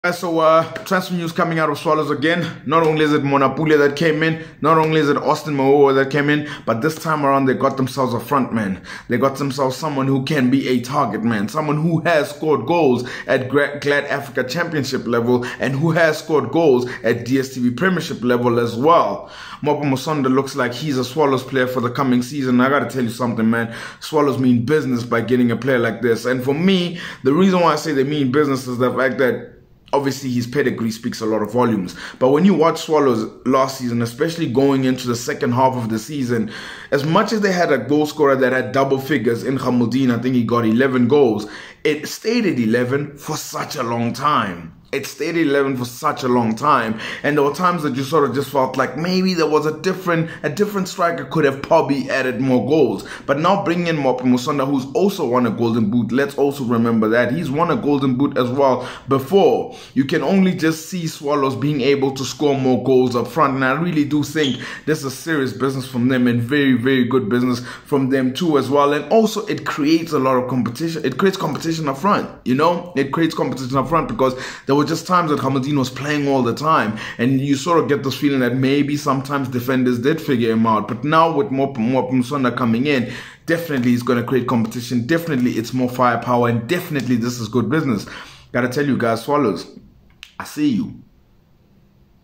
Swallows FC getting Mwape Musonda is serious business
Proven goalscorer too

Will provide the needed competition up front for the club